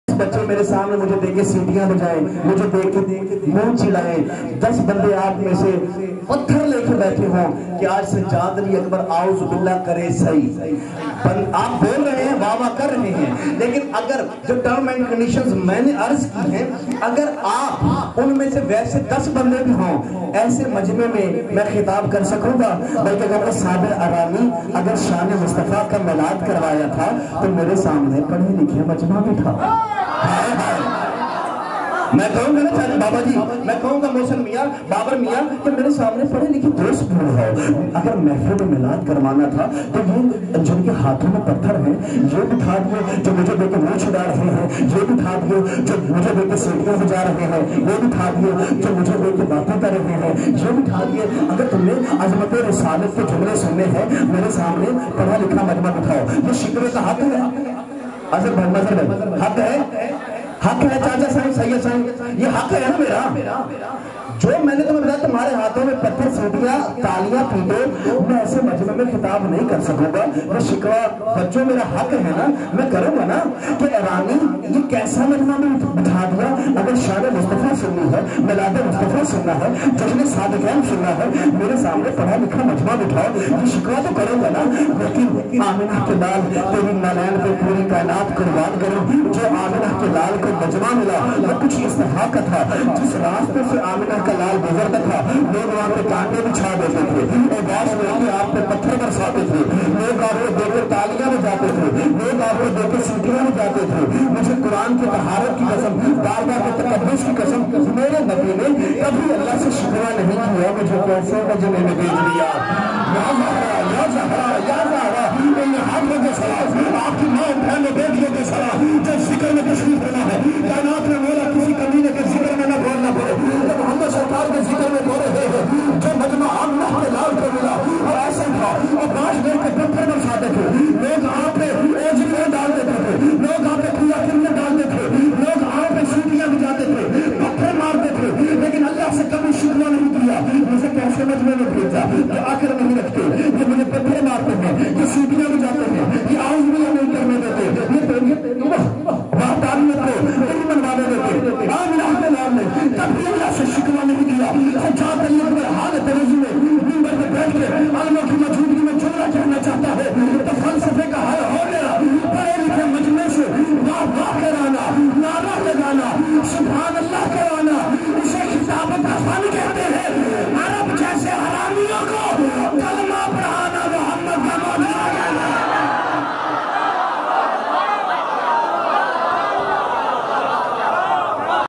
Allama Sajjad Ali Akbar *🌹16 ربیع الاول جشنِ صادقین🌹* امام بارگاہ حسن مجتبیٰ علیہ السلام
*🌹16 ربیع الاول جشنِ صادقین🌹*  امام بارگاہ حسن مجتبیٰ علیہ السلام چک نمبر 215 رب نیھٹری فیصل آباد